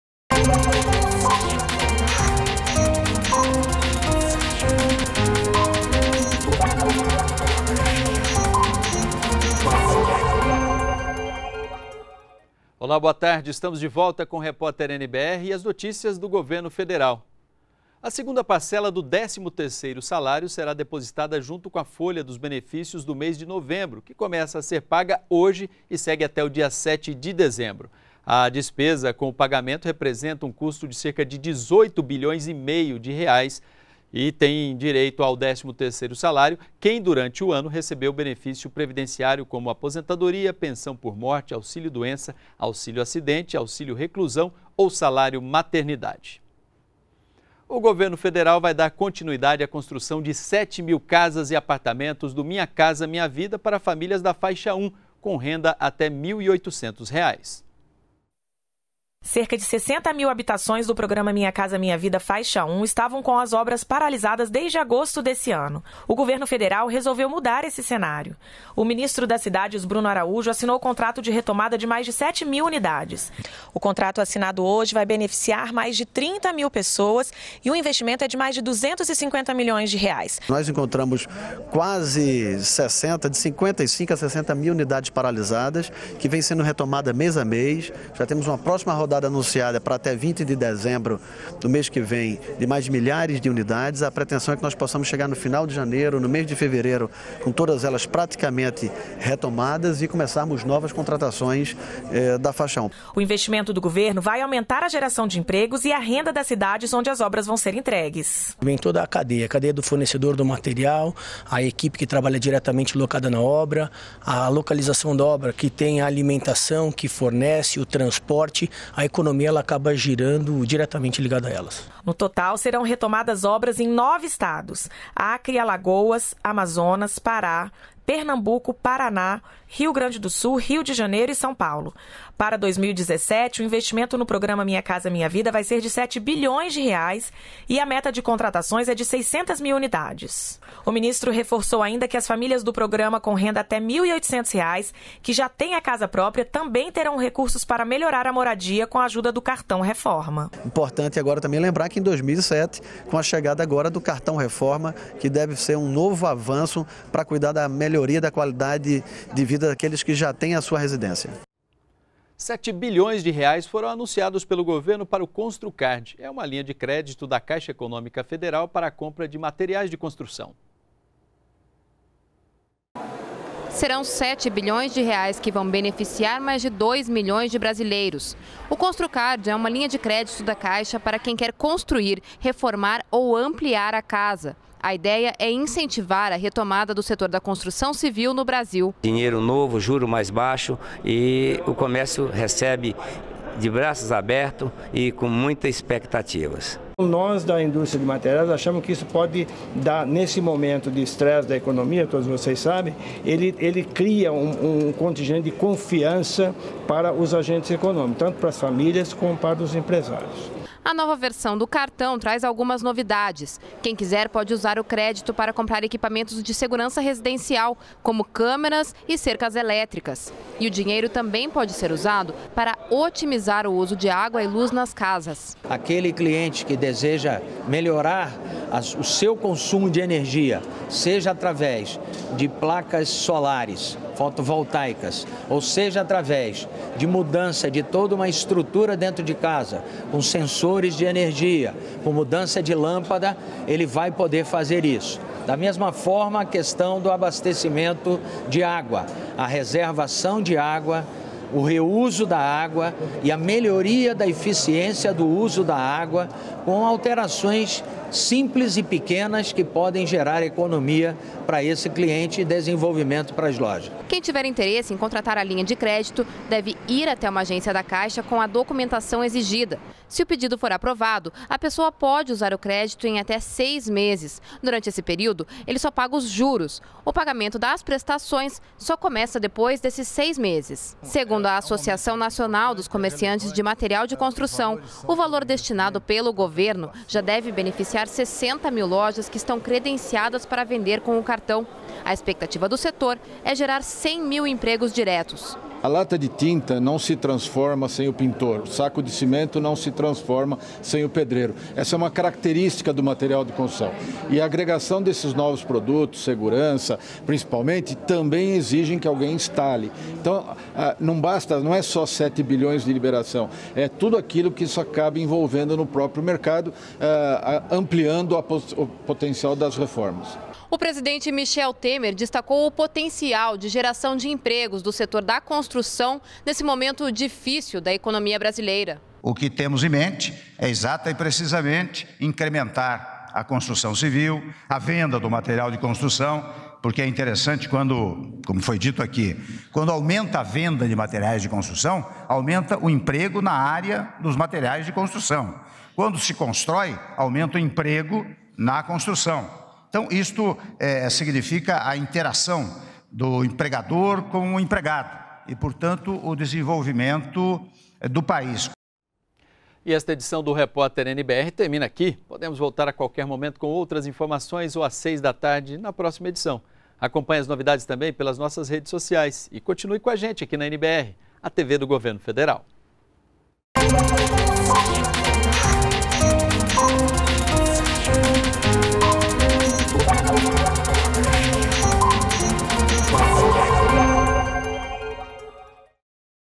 Minha Casa Minha Vida dá continuidade à construção de 7 mil moradias
REPÓRTER NBR 17H - 24.11.16: Governo dá continuidade à construção de 7 mil casas e apartamentos do Minha Casa Minha Vida para famílias da faixa 1, com renda até R$ 1.800,00. Ainda: Caixa  Econômica Federal anuncia R$ 7 bilhões de reais para linha de crédito Construcard.